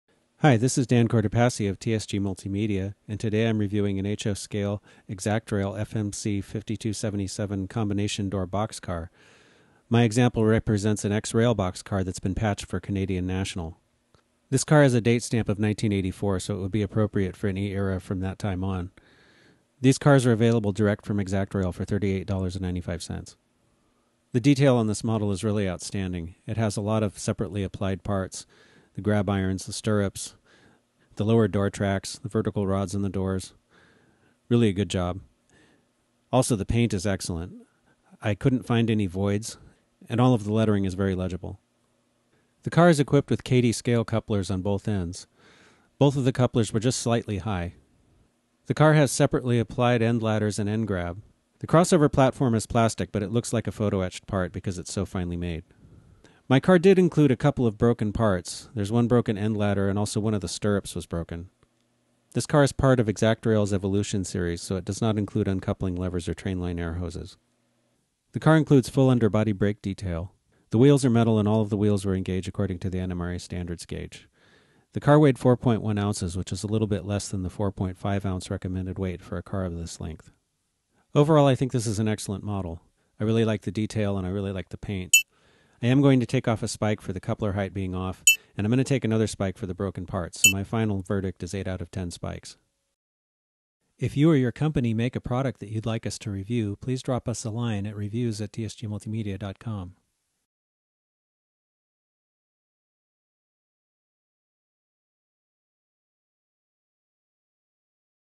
HO Scale Box Car ExactRail Product Review
Up on the table this week, InterMountain's patched RailBox box car! See how it rates on our exclusive Golden Spike Rating System™!